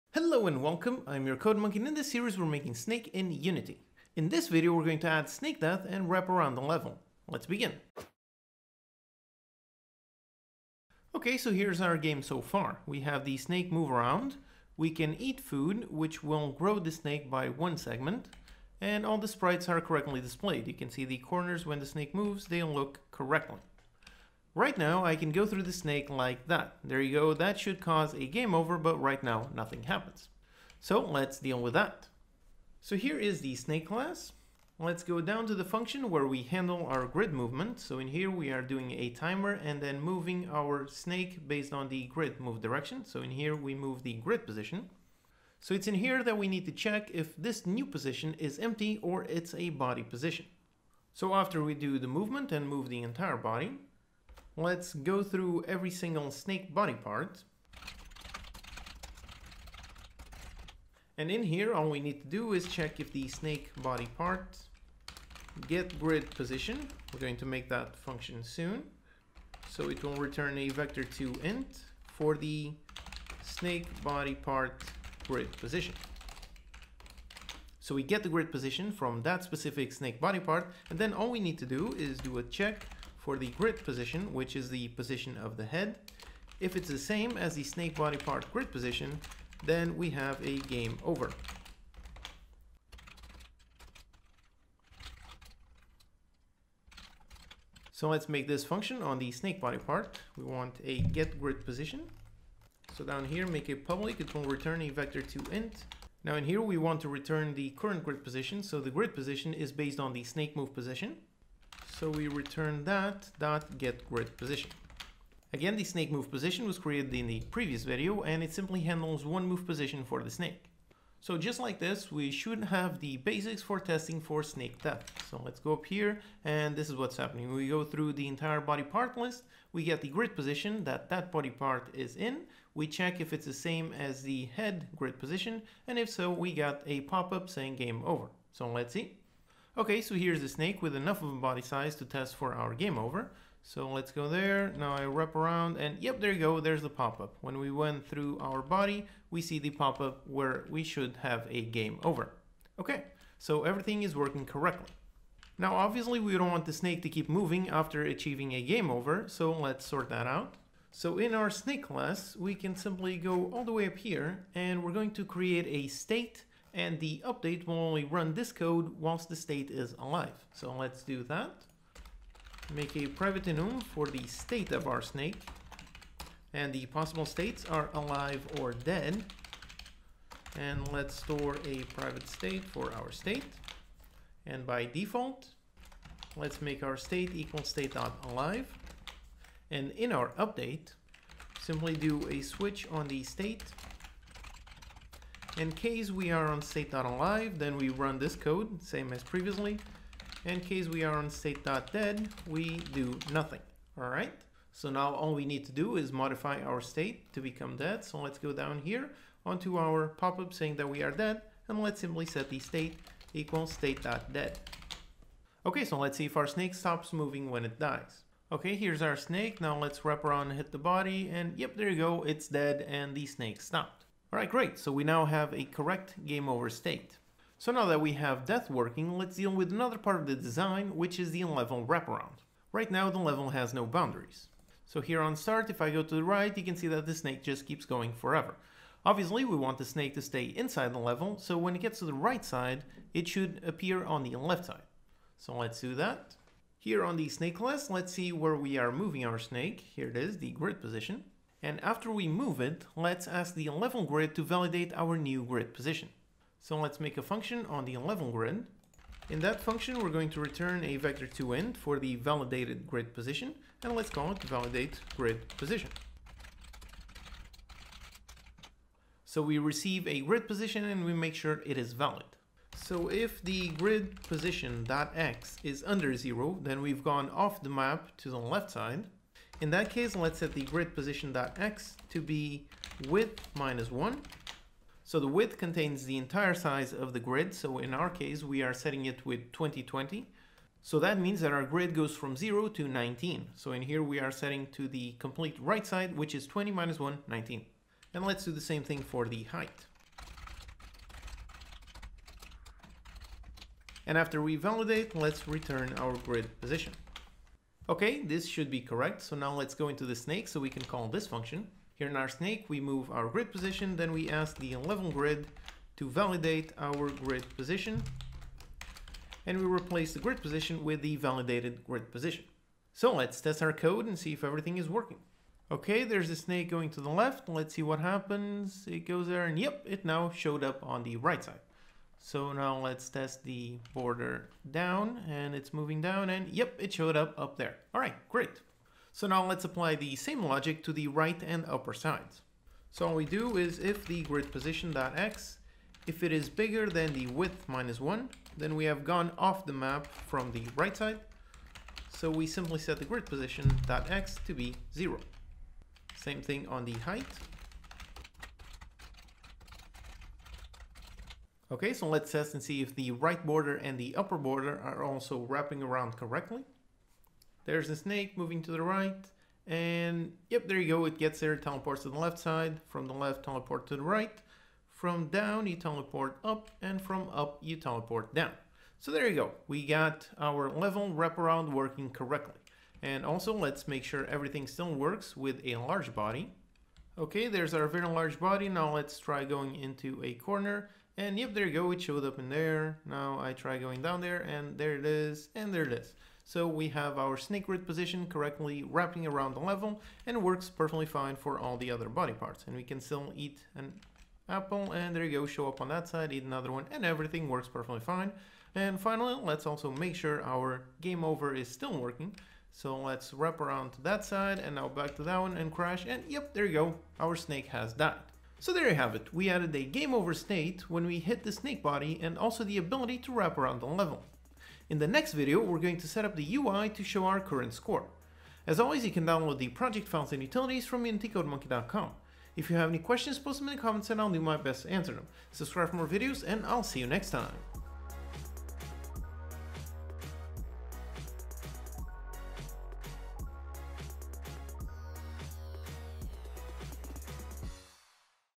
Making Snake in Unity: Death + Wrap (Unity Tutorial for Beginners)
Let's handle Snake Death and Wrap around the Level sides.